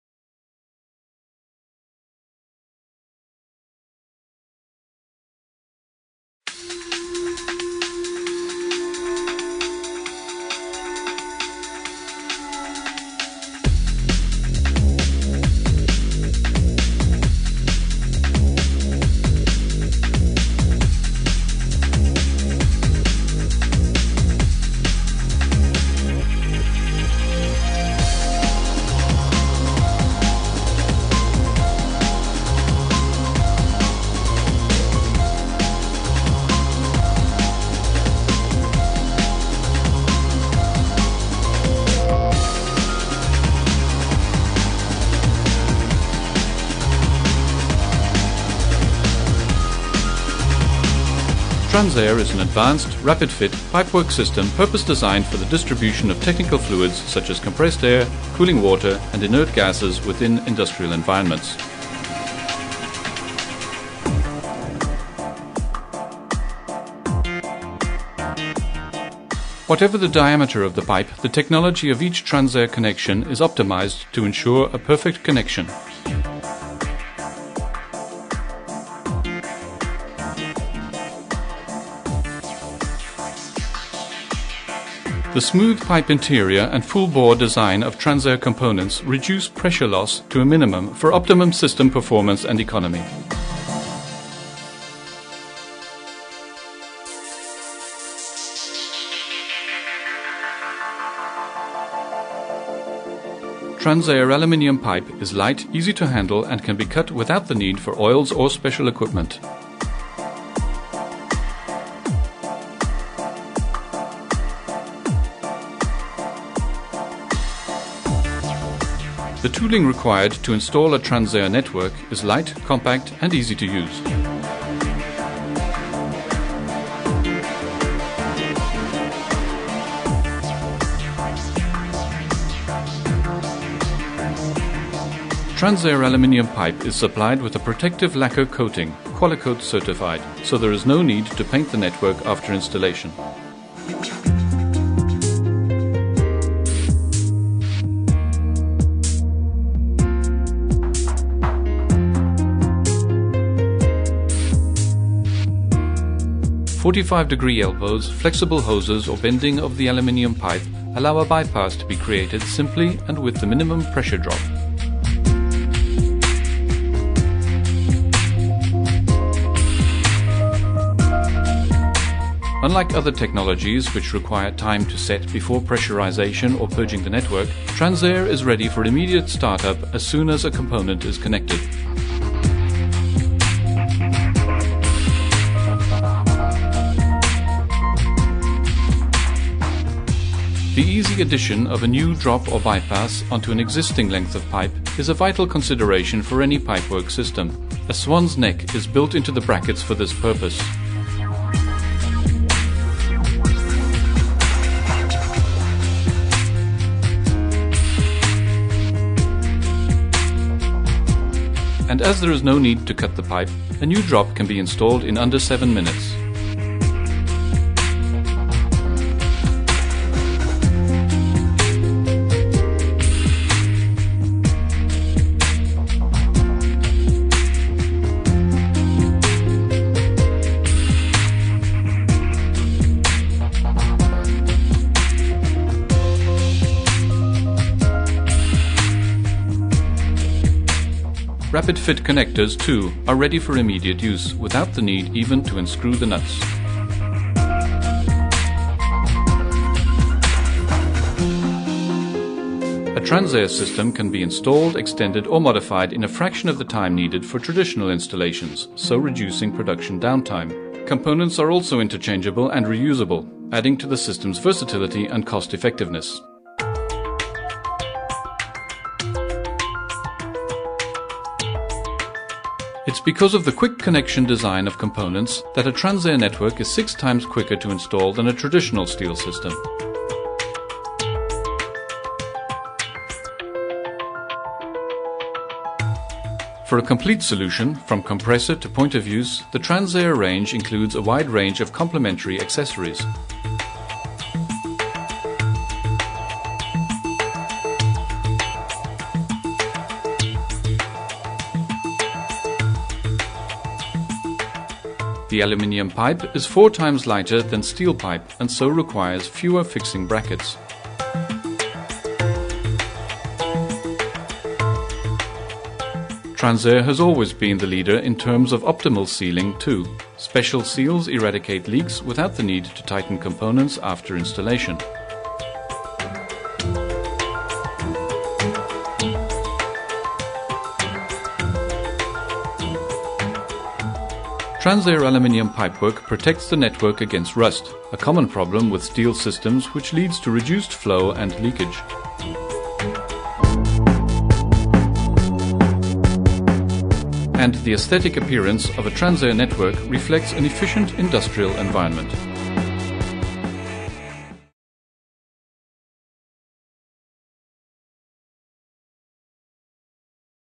Parker Transair - Advanced Air Pipe System
Transair offers the best solutions for all of your compressed air, liquid and inert gas applications. Quick assembly, durability, superior performance, reduced running costs and maximum energy efficiency.
Transair Solution:
• Quick assembly
• Durable Equipment, Clean Air Quality
• Optimum efficiency for machinery and tools
• Overall savings: reduction of installation, operating and commissioning costs
• Certification and Guarantee